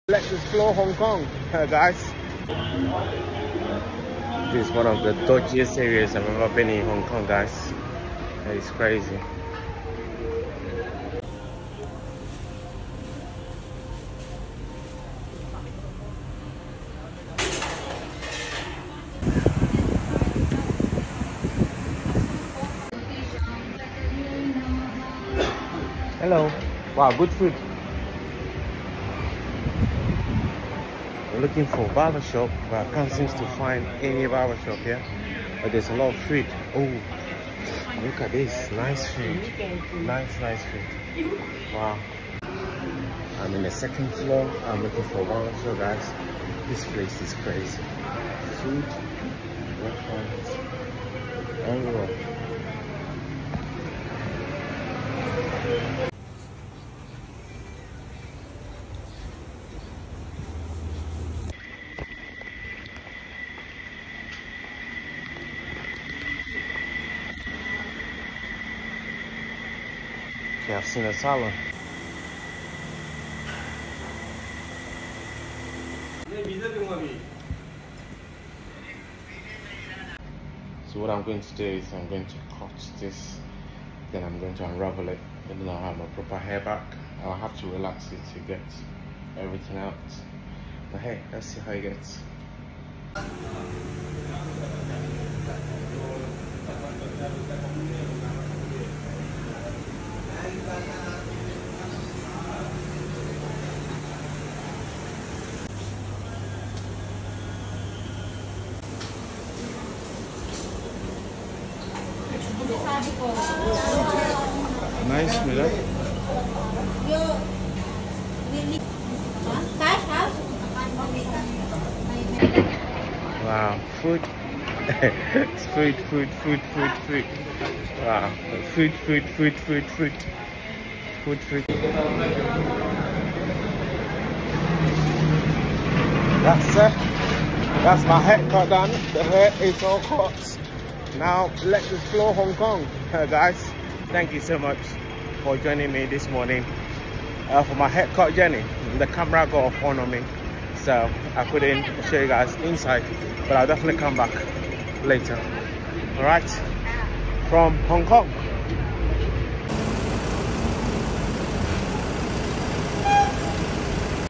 Chungking Mansions 🇭🇰 HONG KONG’S MOST NOTORIOUS AND DODGIEST BUILDING
I explore Hong Kong’s Most Notorious And Dodgiest Building, Chungking Mansions.
In this video, I explore Hong Kong Most Notorious And Dodgiest Building. The building is called Chungking Mansions is a building located at 36–44 Nathan Road in Tsim Sha Tsui, Kowloon, Hong Kong